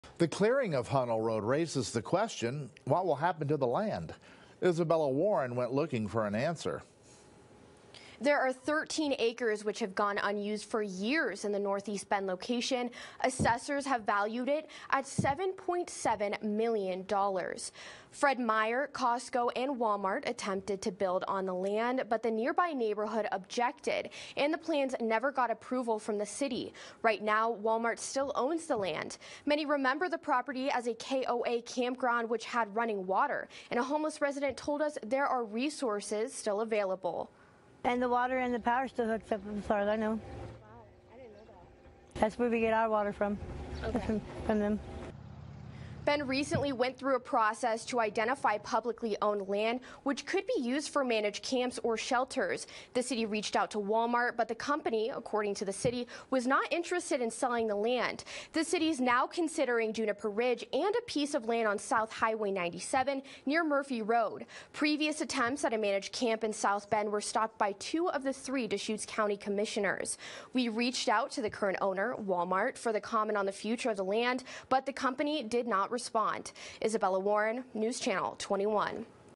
13 acres of land next to Hunnell Road left unused, worth $7.7 million
The Hunnell Road sweep has left many wondering what is happening to the land. Costco, Walmart, and even Fred Meyer have attempted to buy the plot in the past, to no avail.